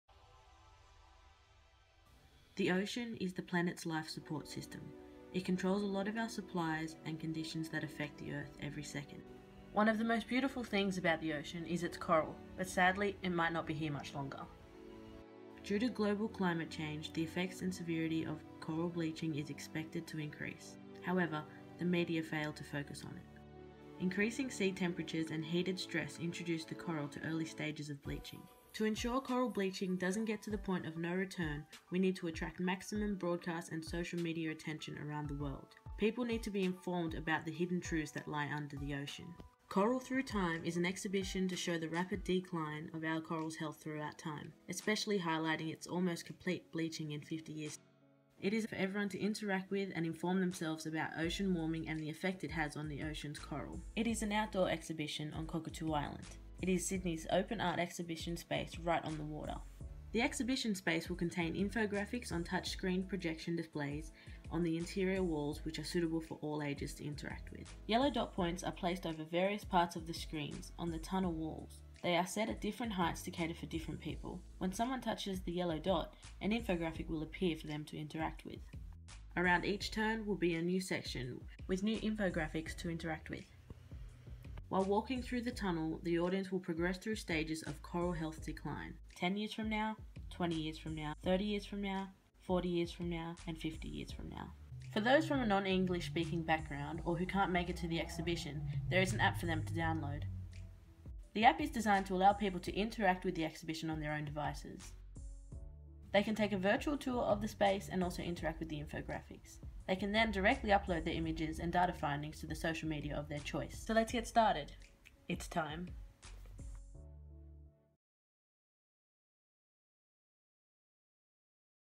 Coral Through Time Assessment 3 z3461020
‘Coral through time’ is an exhibition to show the rapid decline of our corals health throughout time. Especially highlighting its almost complete
bleaching in 50 years time.
It is a nighttime exhibition for everyone to interact with and inform themselves about ocean warming and the affect it has on the oceans coral. The exhibition space will contain infograhics on touch screen projection displays on the interior walls which are suitable for all ages to interact with.
The tunnel like space will also be an infographic itself. While walking through the tunnel the audience will progress through stages of coral health decline. Around each turn, will be a new section which will reflect a decade into the future all the way up to 50 years from now.